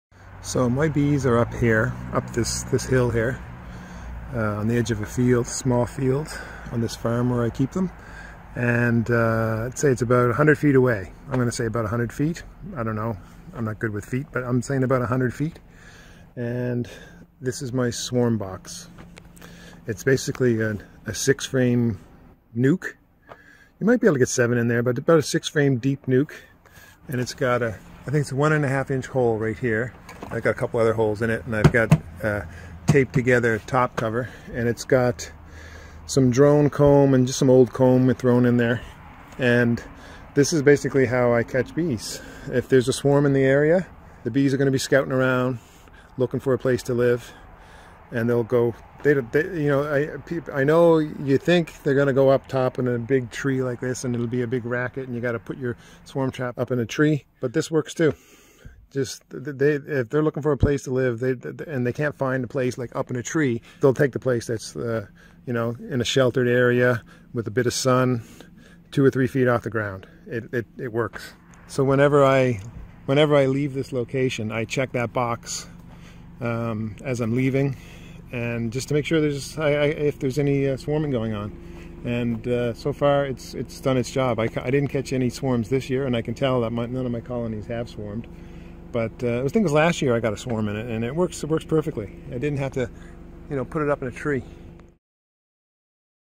Lazy Swarm Box Method
I don't have time to climb up trees to set up swarm traps. I just leave a swarm box about 100 feet south of my hives a few feet off the ground and it works. I know it's not the best method out there, but people who can't climb trees don't need to feed bad.